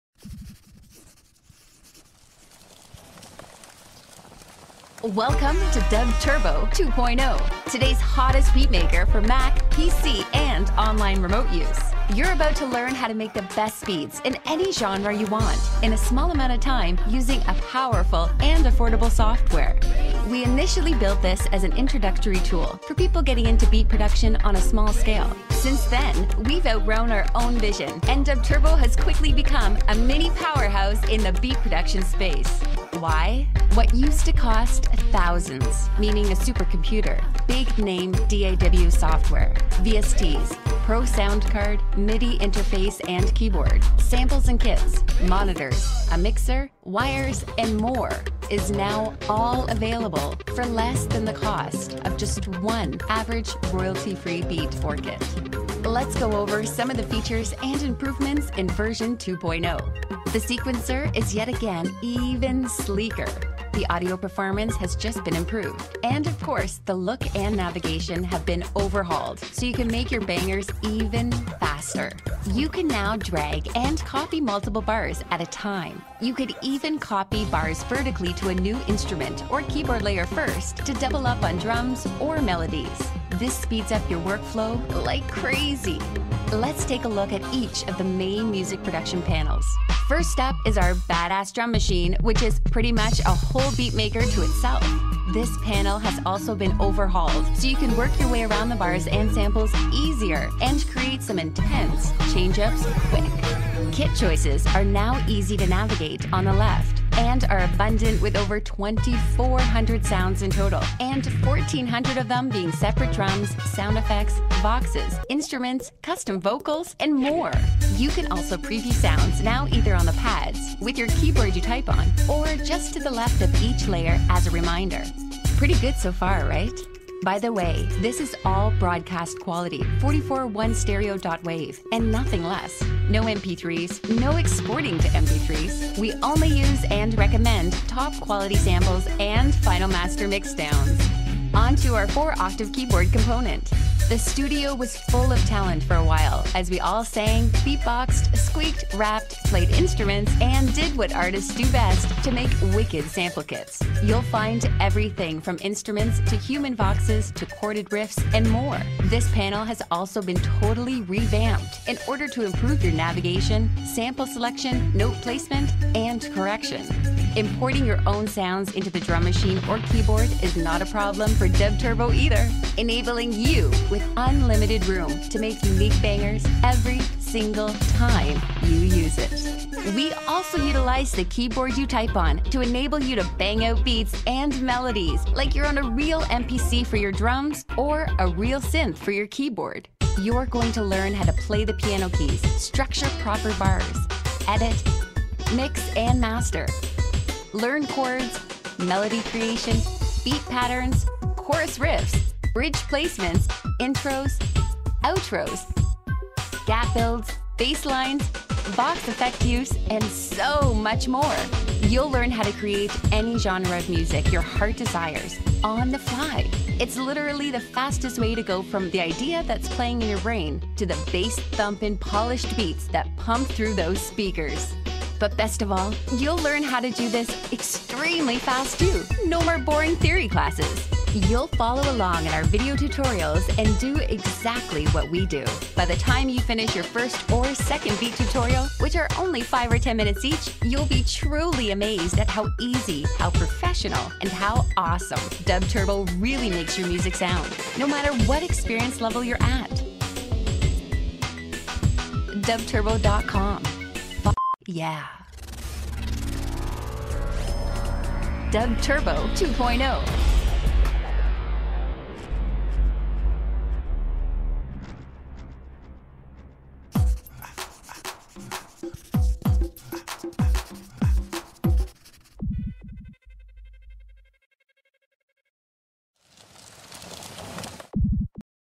Programas Para Mezclar Y Crear Musica
Programas para crear y mezclar musica
El nuevo video para crear Beats usando la tecla de computadora BTV Solo.